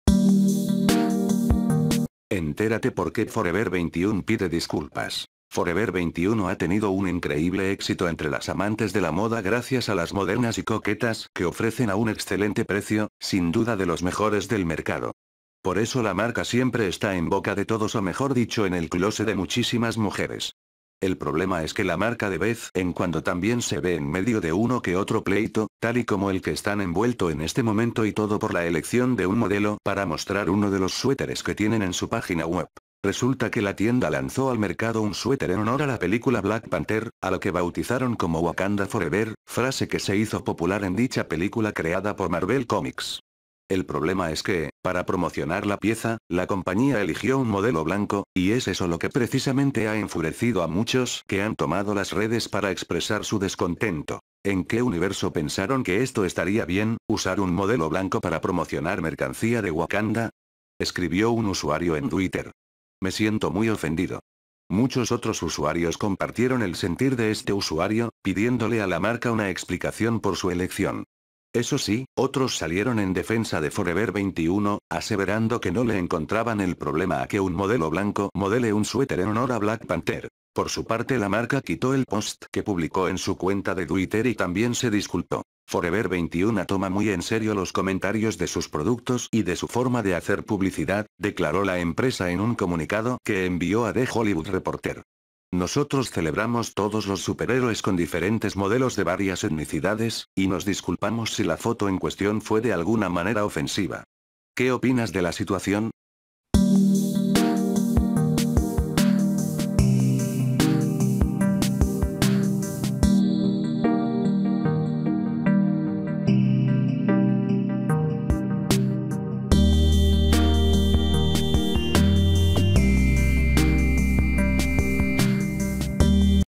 Entérate por qué Forever 21 pide disculpas
Forever 21 ha tenido un increíble éxito entre las amantes de la moda gracias a las modernas y coquetas que ofrecen a un excelente precio, sin duda de los mejores del mercado. Por eso la marca siempre está en boca de todos o mejor dicho en el clóset de muchísimas mujeres. El problema es que la marca de vez en cuando también se ve en medio de uno que otro pleito, tal y como el que están envuelto en este momento y todo por la elección de un modelo para mostrar uno de los suéteres que tienen en su página web.